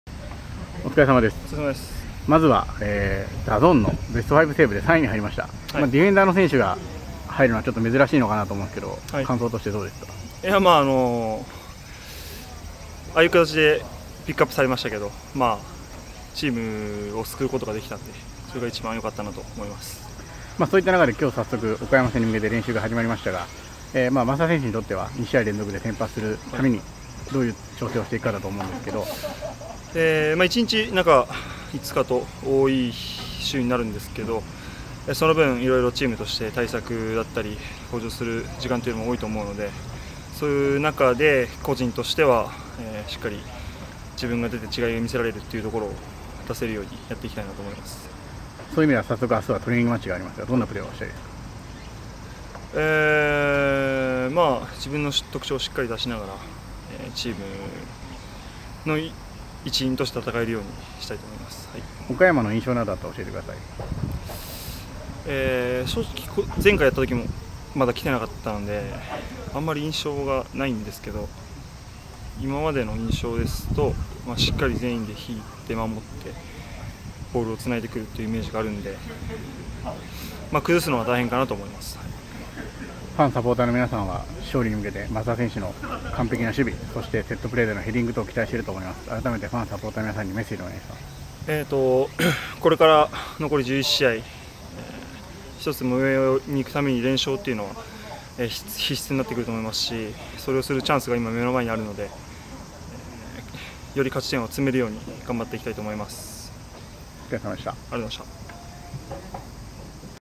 9月5日トレーニング後の増田繁人選手コメントです！
9月5日のトレーニング後の増田繁人選手コメントです！
皆様ぜひ、御覧ください！